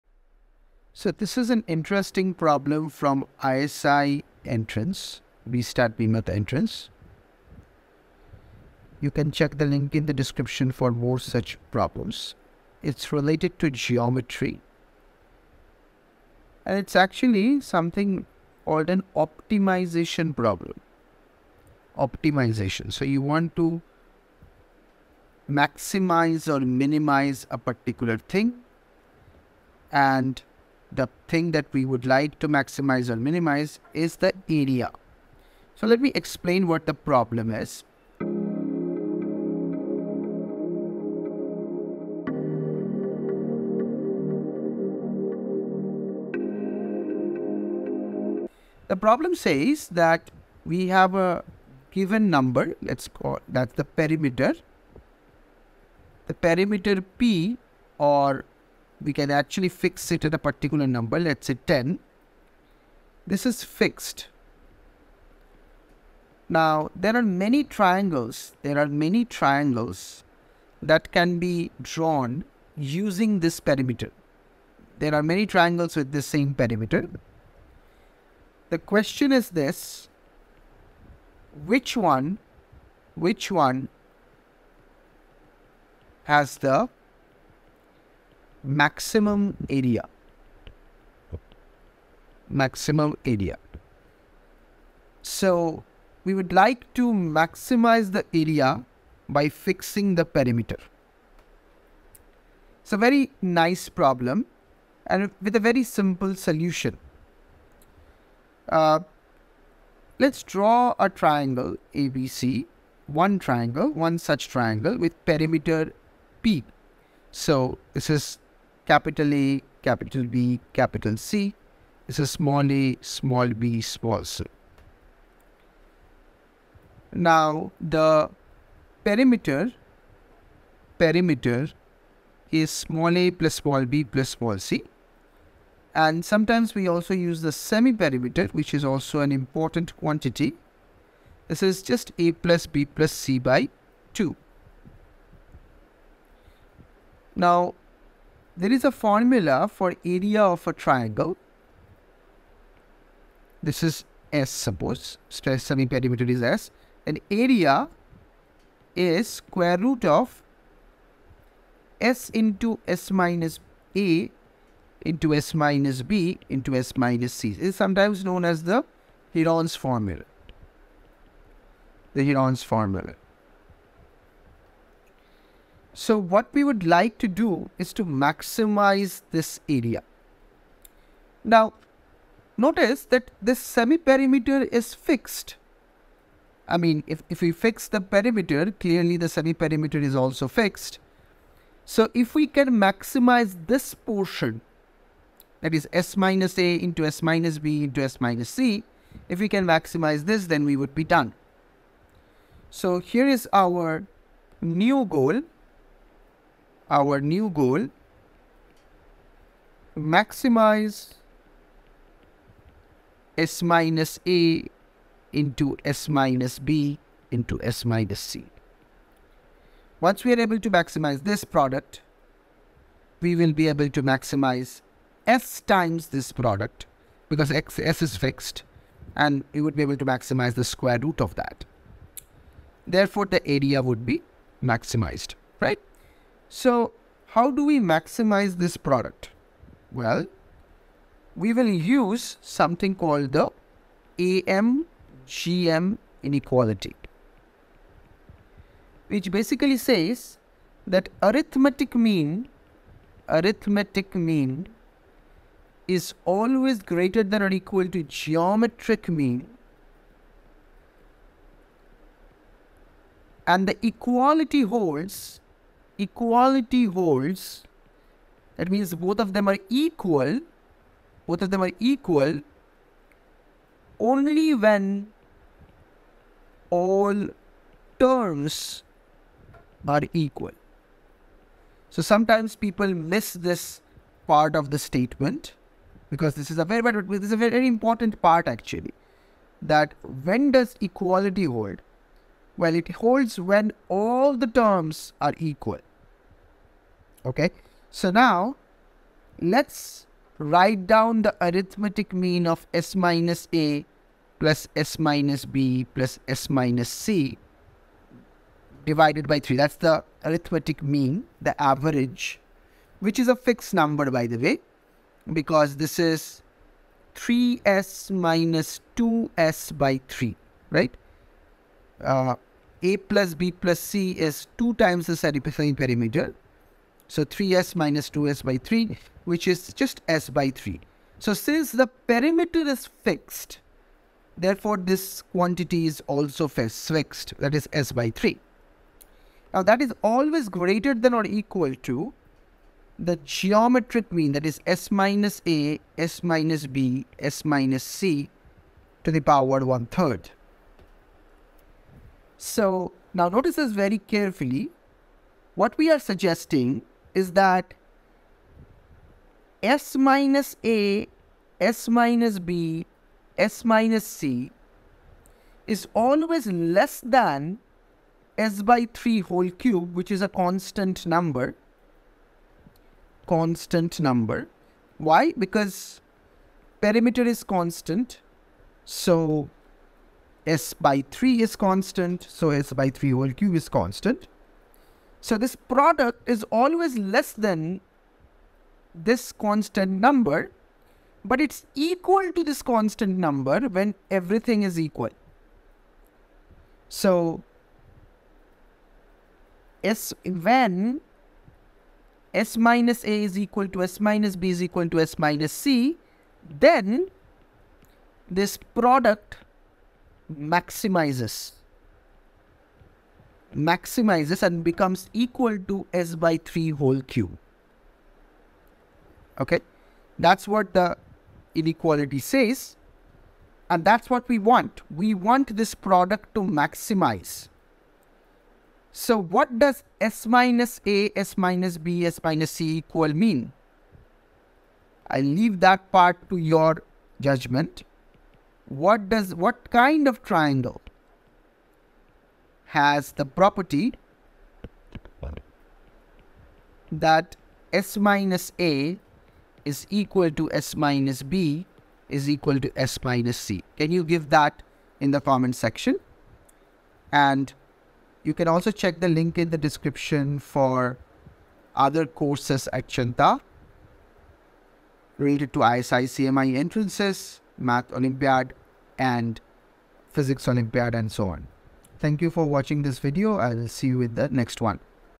How to maximize area of a triangle with given perimeter | BStat 2008 Subjective Problem 1
Join cheenta.com for outstanding Math Olympiad, ISI-CMI Entrance Program.